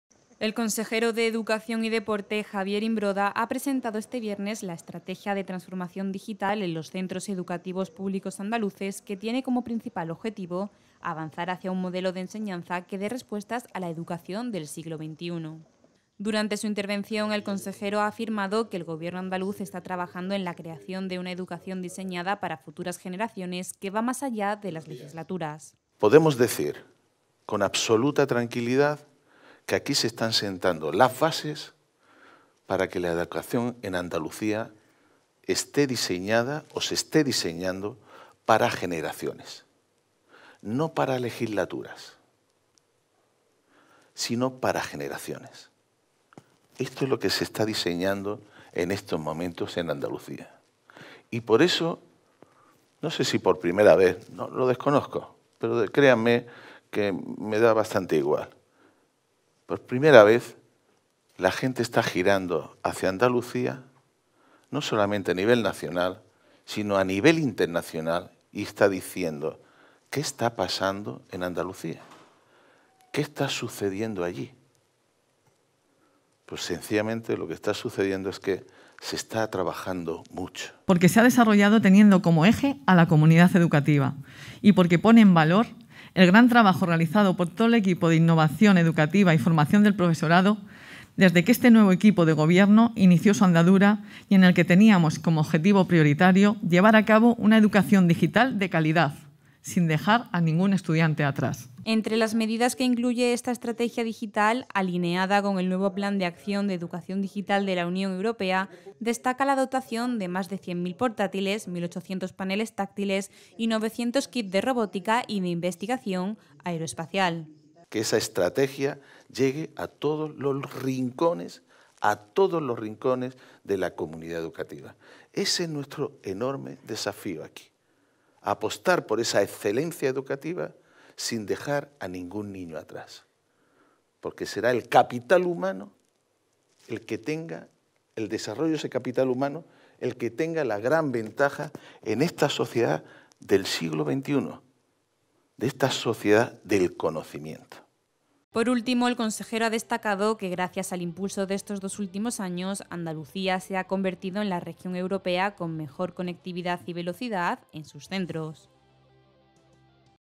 La Junta presenta la Estrategia de Transformación Digital en los centros educativos andaluces
El consejero de Educación y Deporte, Javier Imbroda, ha presentado este viernes la Estrategia de Transformación Digital en los centros educativos públicos andaluces, que tiene como objetivo avanzar hacia un modelo de enseñanza que dé respuestas a la educación del siglo XXI.
Durante su intervención, el consejero ha afirmado que el Gobierno andaluz está trabajando en la creación de una educación diseñada para futuras generaciones que va más allá de las legislaturas.
Entre las medidas que incluye esta estrategia digital, alienada con el nuevo Plan de Acción de Educación Digital de la Unión Europea, destaca la dotación de más de 100.000 portátiles,  1.800 paneles táctiles y 900 kits de robótica y de investigación aeroespacial.